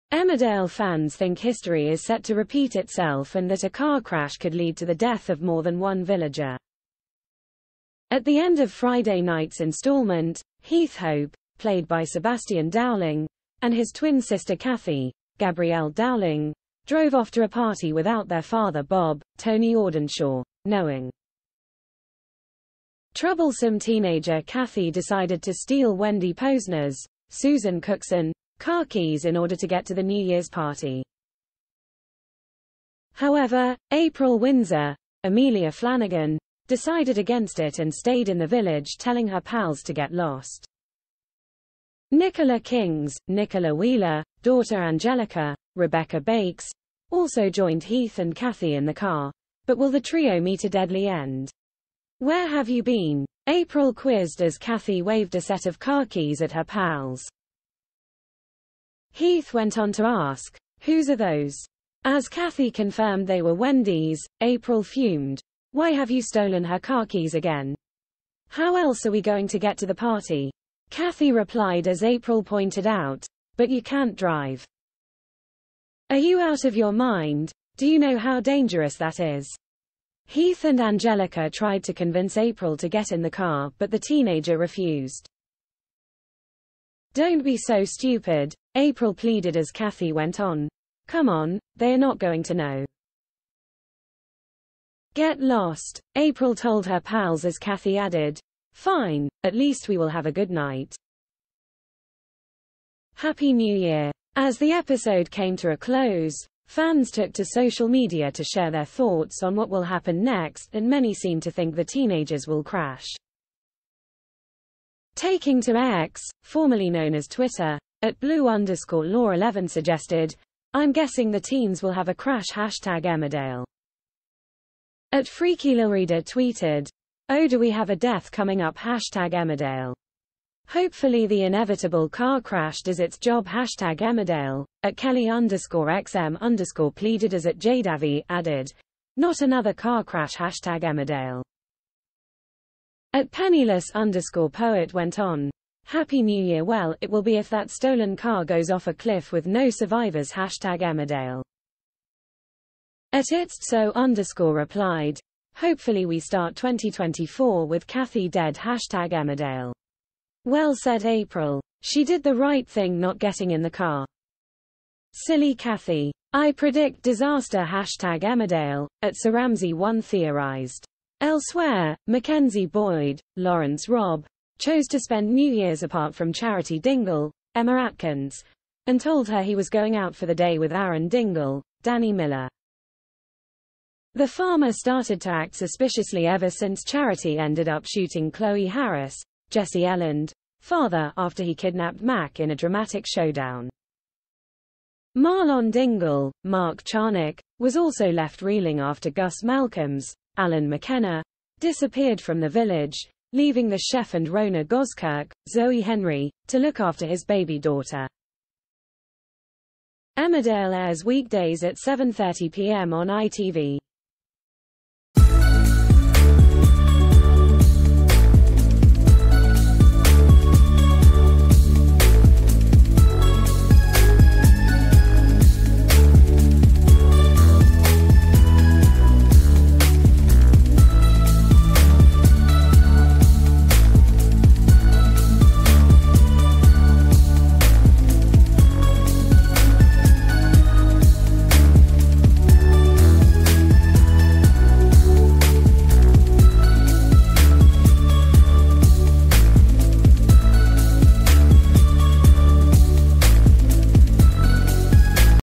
Emmerdale exit as fans predict 'disaster' for three beloved villagers
Emmerdale fans think history is set to repeat itself and that a car crash could lead to the death of more than one villager. At the end of Friday night's instalment, Heath Hope (played by Sebastian Dowling) and his twin sister Cathy (Gabrielle Dowling) drove off to a party without their father Bob (Tony Audenshaw) knowing. Troublesome teenager Cathy decided to steal Wendy Posner's (Susan Cookson) car keys in order to get to the New Year's Party. However, April Windsor (Amelia Flanagan) decided ...